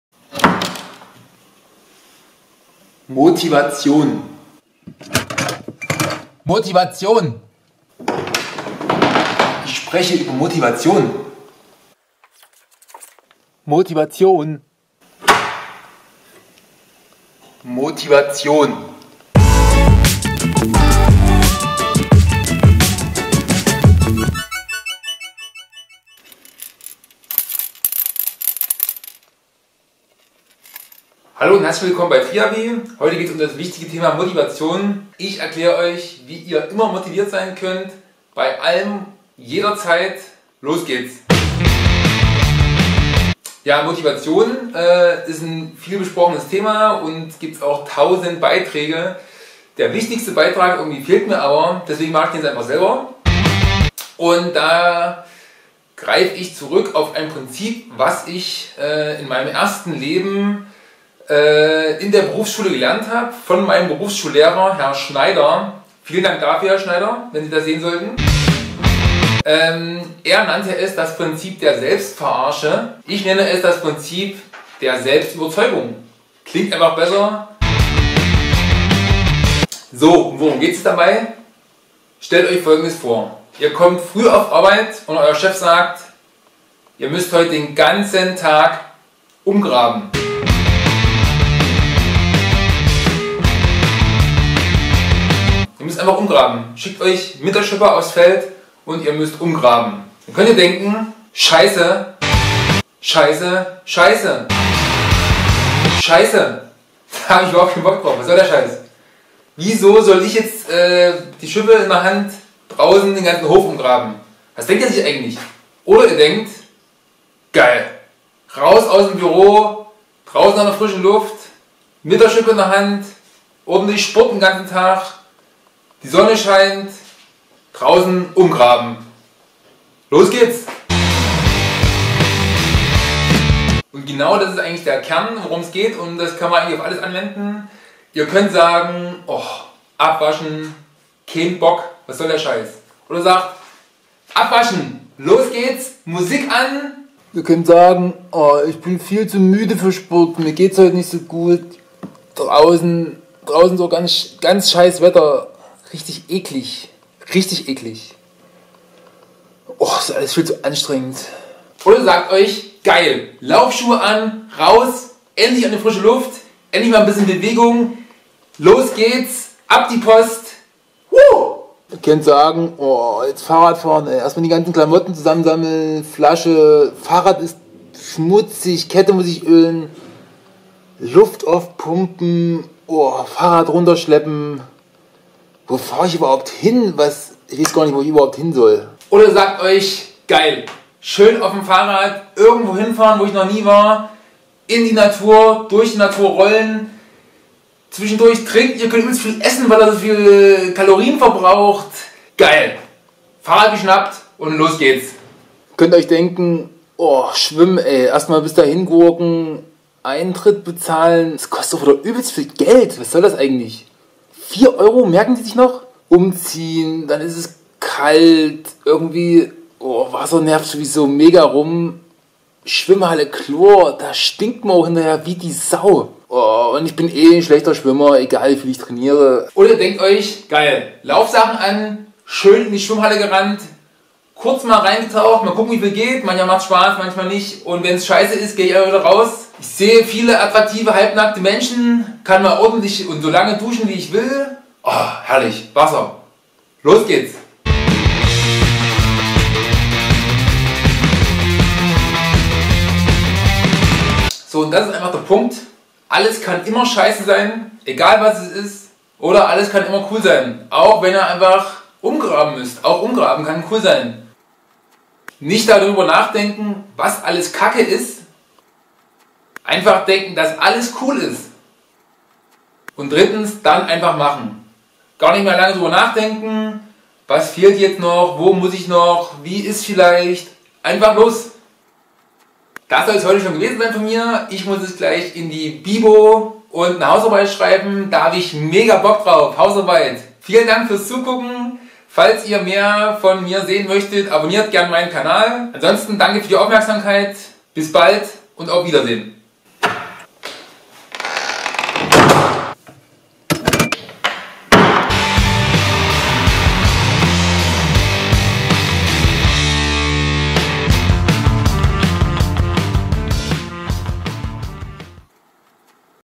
Triaby #14 -Motivation für Triathlon, Fitness, Sport und Umgraben- Motiviere dich selbst!
Motivation! Alle wollen es! Ich zeige wie das geht, mit der Motivation.

Egal ob du Triathlon oder Fitness machst, ob du aufräumen musst oder Umgraben. Es ist eigentlich ganz einfach sich zu motivieren!